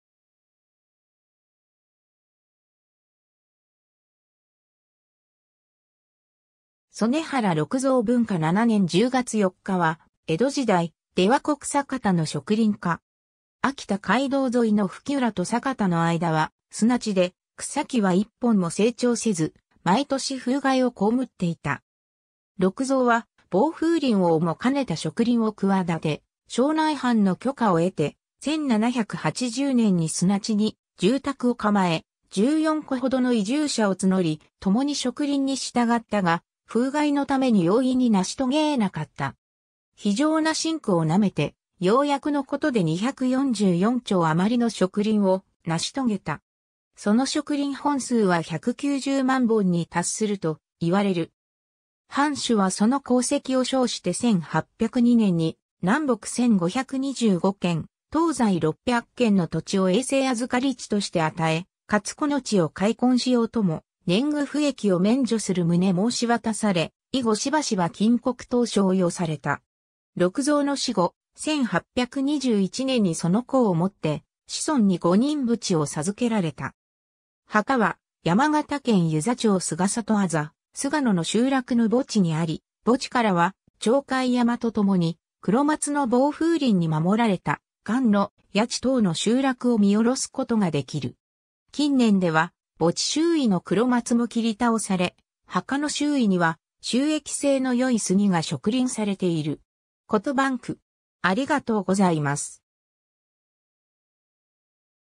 曾根原六蔵
曾根原 六蔵（そねはら ろくぞう、寛保3年（1743年） - 文化7年10月4日（1810年10月31日））は、江戸時代、出羽国酒田の植林家。
 秋田街道ぞいの吹浦と酒田の間は、沙地で草木は一本も生長せず、毎年風害をこうむっていた。
六蔵は防風林をも兼ねた植林をくわだて、庄内藩の許可をえて、1780年（安永9年）に沙地に住宅をかまえ、14戸ほどの移住者を募り、ともに植林に従ったが、風害のために容易に成し遂げえなかった。
非常な辛苦をなめてようやくのことで244町余の植林を成し遂げた。
その植林本数は190万本に達すると言われる。
藩主はその功績を賞して1802年（享和2年）に南北1525間、東西600間の土地を永世預り地として与え、かつこの地を開墾しようとも年貢賦役を免除する旨申し渡され、以後しばしば金穀等賞与された。
六蔵の死後、1821年（文政4年）にその功をもって子孫に5人扶持を授けられた。
墓は山形県遊佐町菅里字菅野の集落の墓地にあり、墓地からは鳥海山と共に黒松の防風林に守られた菅野、谷地等の集落を見下ろすことができる。
 近年では墓地周囲の黒松も切り倒され、墓の周囲には収益性のよい杉が植林されている。
 ^ コトバンク